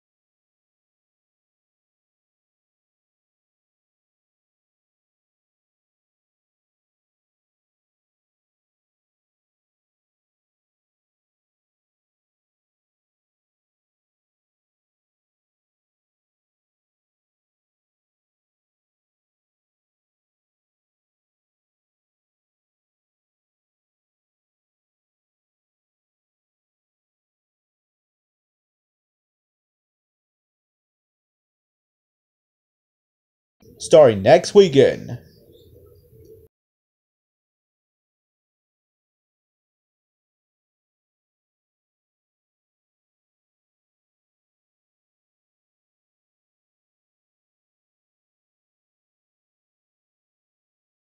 Random Stuff #54: A Special Surprise for everyone!
This 3 days ago (which is August 2), my dad brought me a another phone (instead of new Tablet), so starting next weekend, I'm going back to make Dream Logo Variations/Combo and of course Opening/Closing Logos, Homemade VHS Opening & others thing i done.

Special Thanks/Credits & Inspired by: Ginger Manley, 7 eleven & James TheCartoonMan.

Tim is a trademark of JR100 Entertainment Media Group Inc.